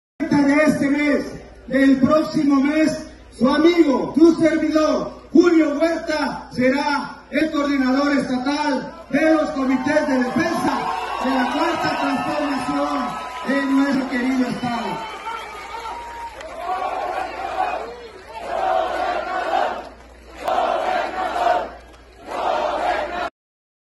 Julio Huerta sostiene una reunión con los residentes de Zinacatepec
Julio Huerta, quien aspira a ser el coordinador de la Cuarta Transformación en Puebla, sostiene una reunión con los residentes de Zinacatepec